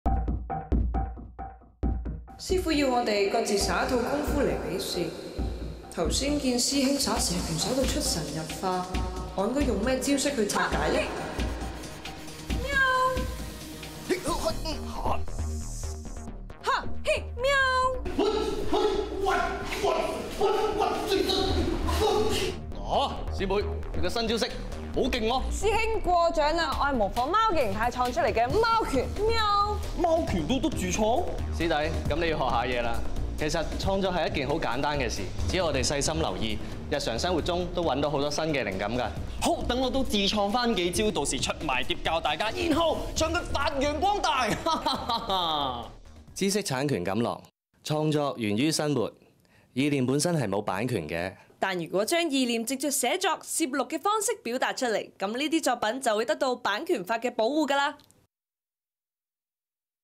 創作源於生活
Launched on 20 Jun 2013

師妹: 師傅要我們各自耍一套功夫來比試，剛才看師兄耍蛇拳耍得出神入化，我應該用甚麼招式去-拆解呢？
師兄: 師妹，你的新招式很厲害！
師妹: 師兄過獎！這是模仿貓的形態而創的貓拳。喵！
師弟: 貓拳也可以自創？
師兄: 師弟，那你要學習一下。其實創作是一件很簡單的事，只要我們細心留意，在日常生活中也-可以找到很多新的靈感。
師弟: 讓我也自創數招，到時候再出光碟教大家﹐然後將它發揚光大！
(熒幕蓋字)知識產權錦囊︰創作源於生活﹐意念本身沒有版權，將意念以寫作、攝錄等方¬式表達，作品便會得到版權保護。